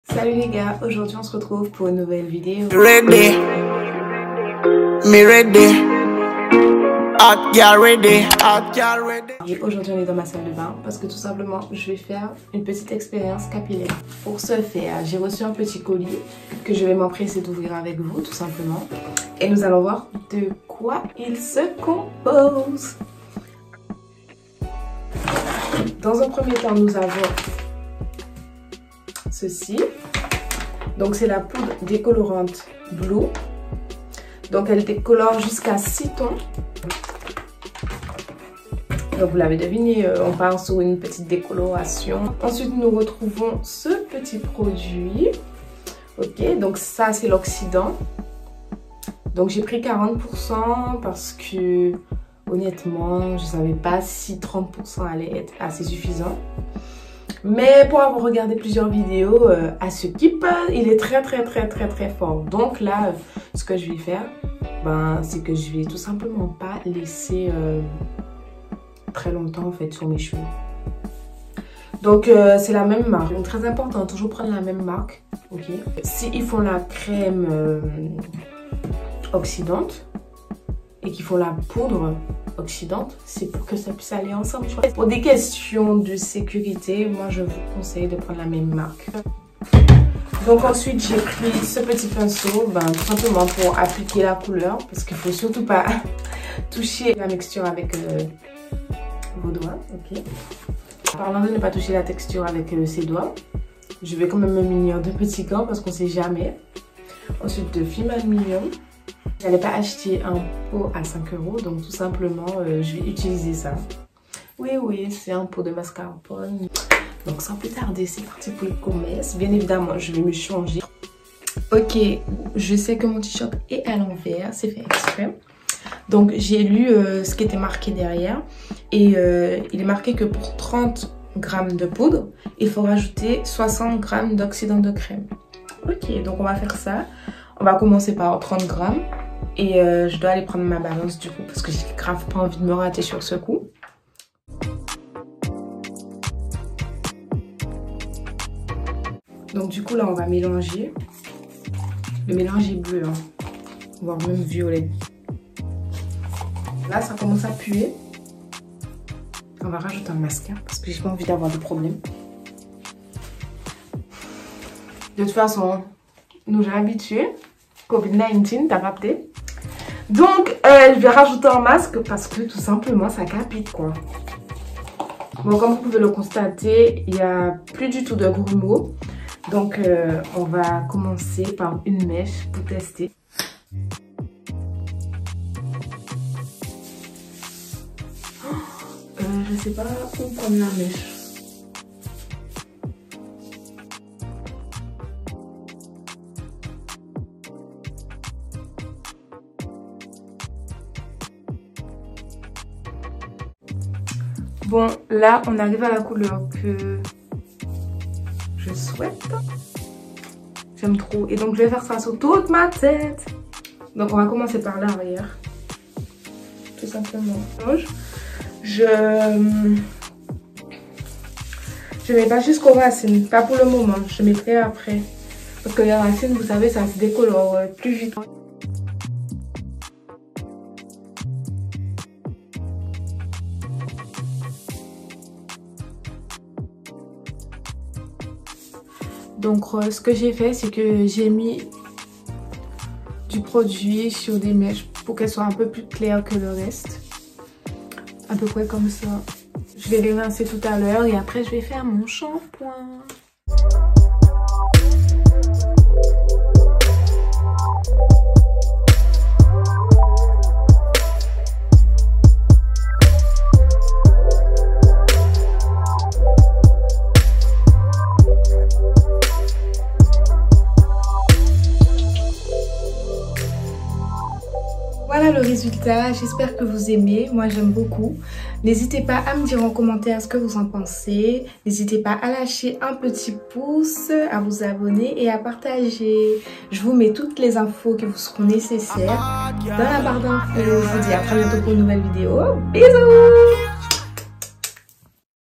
Je décolore mes cheveux Naturels 🙊
Salut les gars ! Aujourd’hui nous partons sur une petite décolo des familles les produits utilisés viennent du site "La plateforme du coiffeur ".

N’ hésitez pas à liker partager et commenter 🪴

Retrouvez-nous sur Instagram :

🍯 :honey_tbh

🦋: dow_glg

Xoxo 💋